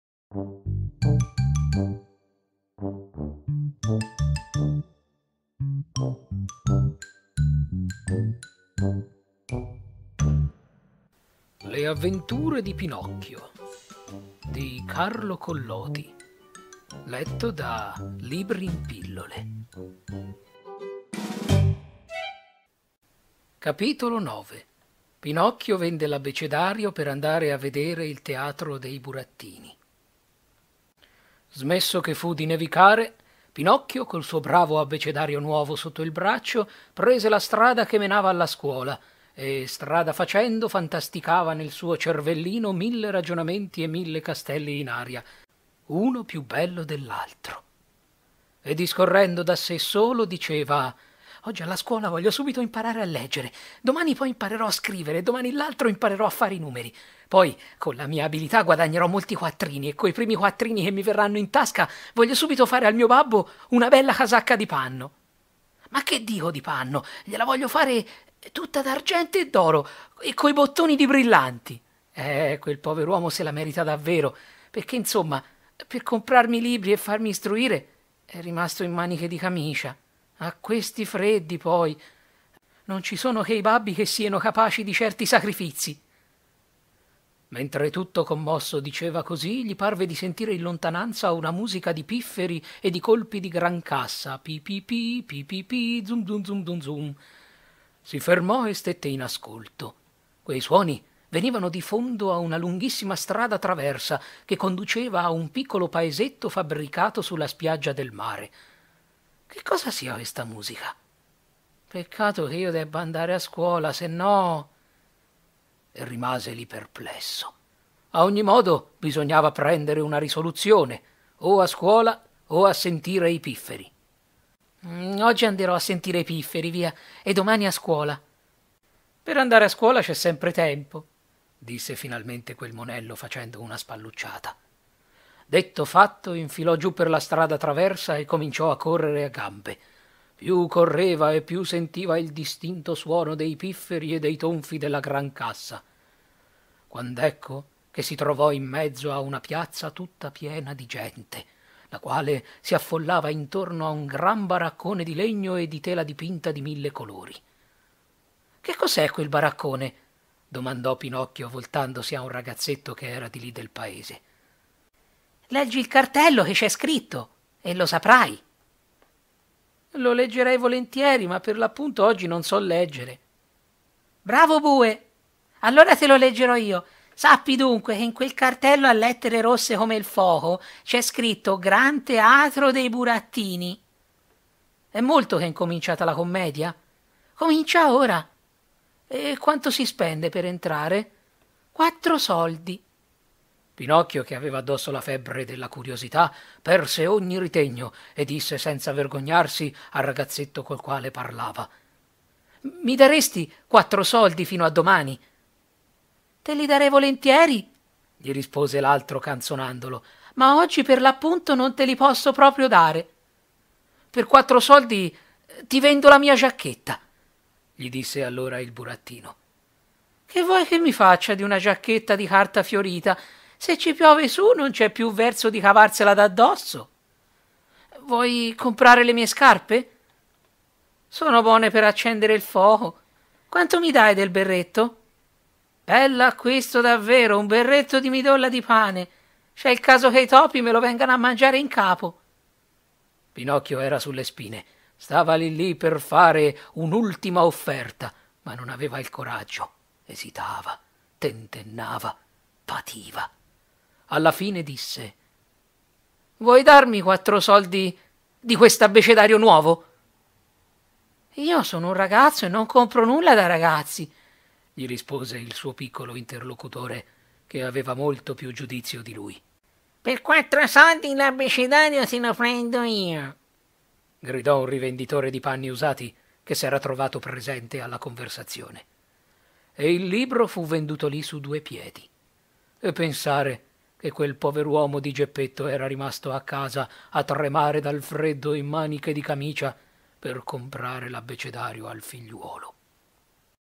Audiolibro C. Collodi: Le Avventure di Pinocchio - Capitolo IX
Pinocchio vende l’Abbecedario per andare a vedere il teatrino dei burattini.

VOCI AGGIUNTE
Mastro Ciliegia, Grillo-Parlante e Gatto: Zartan

Fonte del Testo: Wikisource con licenza Creative Commons Attribuzione - Condividi allo stesso modo 3.0 Unported